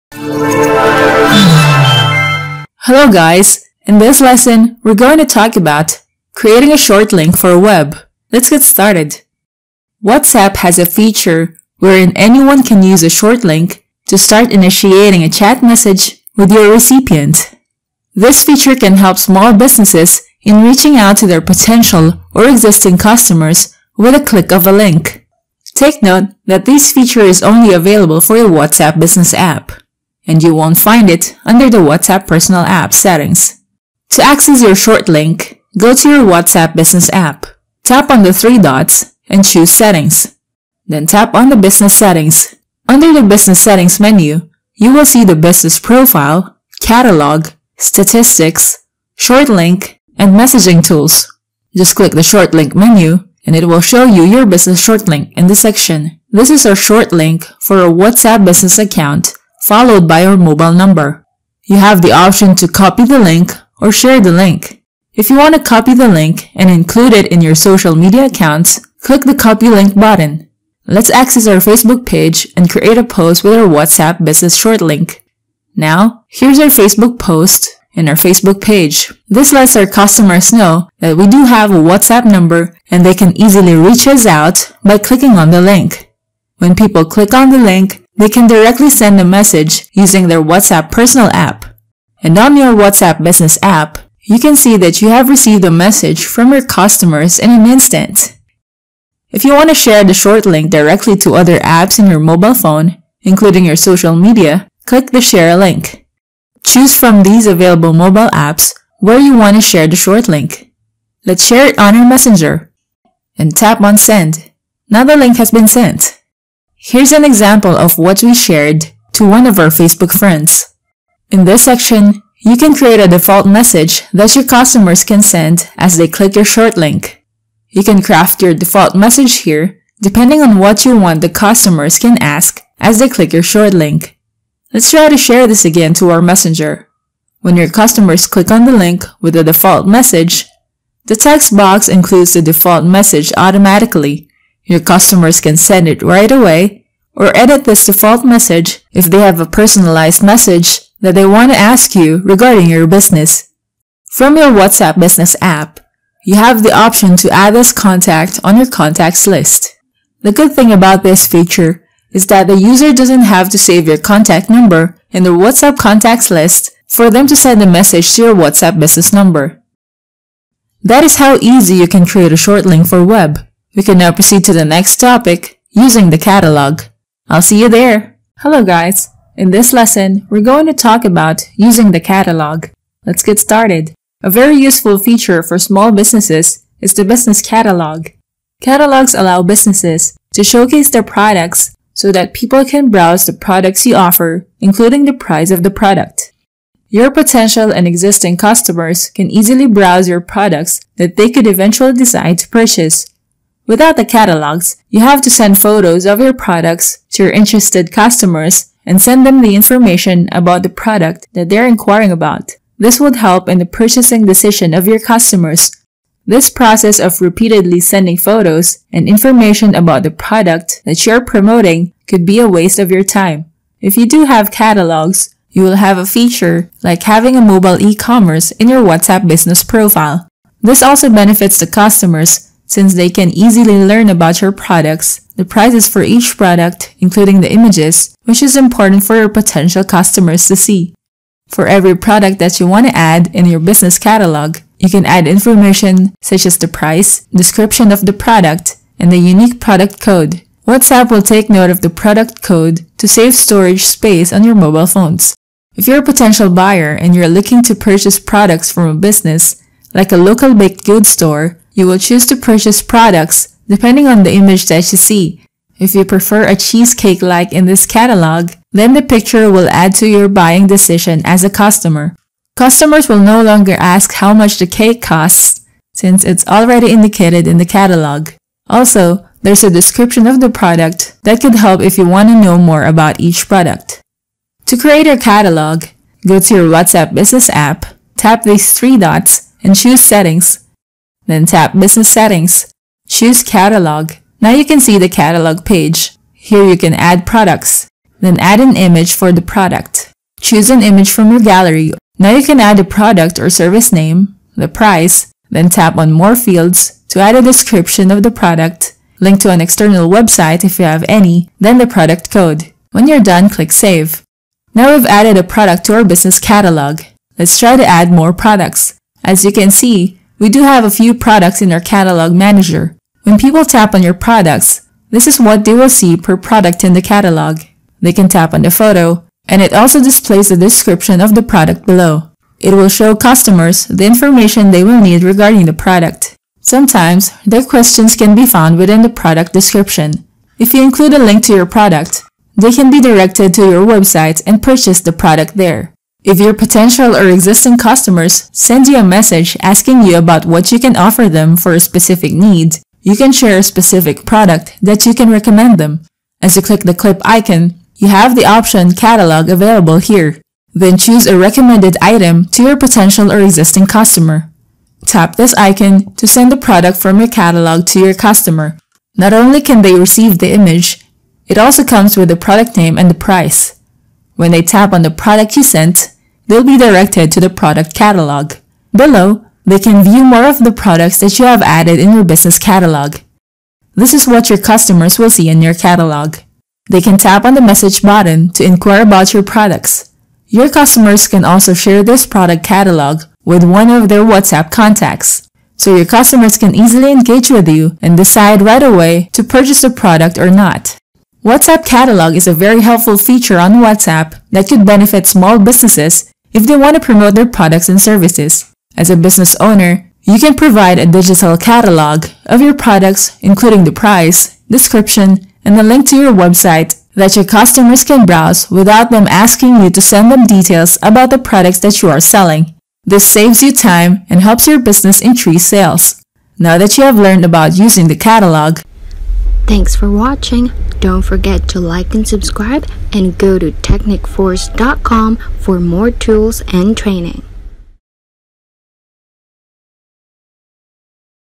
Selling Products On Whatsapp Using Links & Catalogs
WhatsApp has a feature wherein anyone can use a short link to start initiating a chat message with the recipient. This feature can help small businesses in reaching out to their potential or existing customers with a click of a link. Take note that this is an only available feature for the WhatsApp Business App and you won't find it under the WhatsApp Personal App.

Another very useful feature for small businesses is the Business Catalog. Catalogs allow businesses to showcase their products so that people can browse the products you offer including the price of the product.

00:00 Introduction
00:01 Create a short link for web
02:56 Using the Catalog